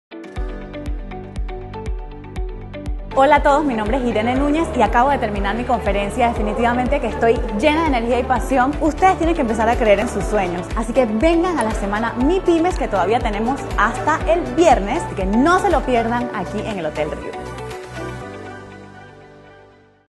Tercera jornada de la edición 11 de Semana Mipyme
SemanaMipyme2017 Buena energía es lo que se respira durante la tercera jornada de la edición 11 de Semana Mipyme. ¡Recárgate con la energía de los emprendedores más exitosos de Panamá!